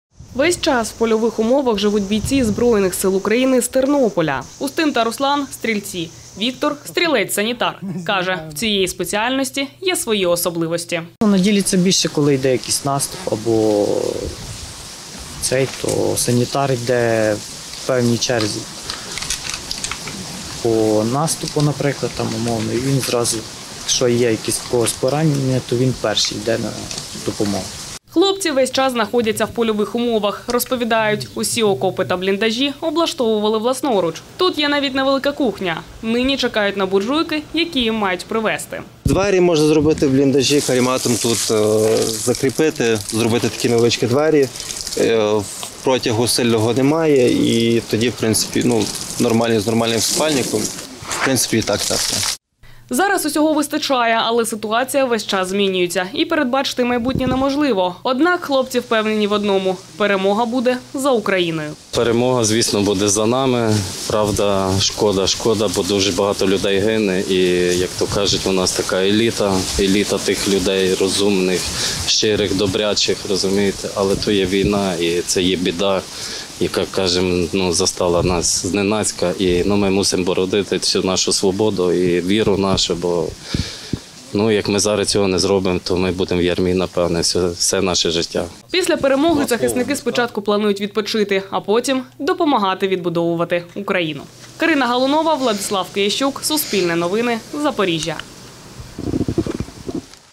На Запорізькому напрямку служать добровольці з Тернополя | Новини
Усі троє в перший день війни пішли до військкомату та були направлені на Запорізький напрямок. Тернополяни Устим, Руслан та Віктор вже п’ять місяців боронять Україну на передовій.